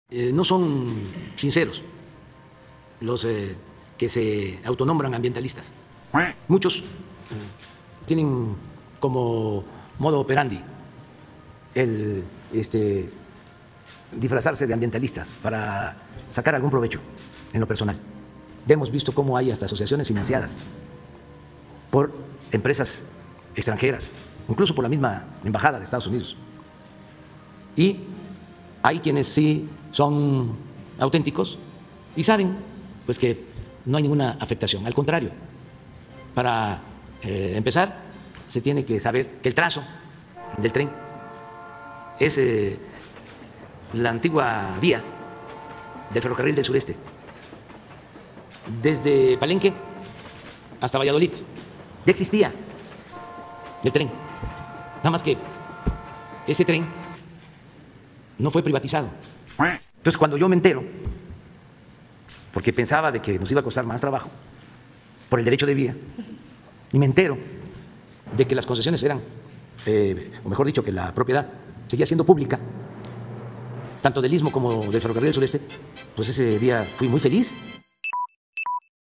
Los auténticos ambientalistas saben que el TrenMaya no causa ninguna afectación 🦆🤥
No son sinceros los que se autonombran ambientalistas, porque los auténticos ambientalistas saben que el TrenMaya no causa ninguna afectación sino todo lo contrario 🦆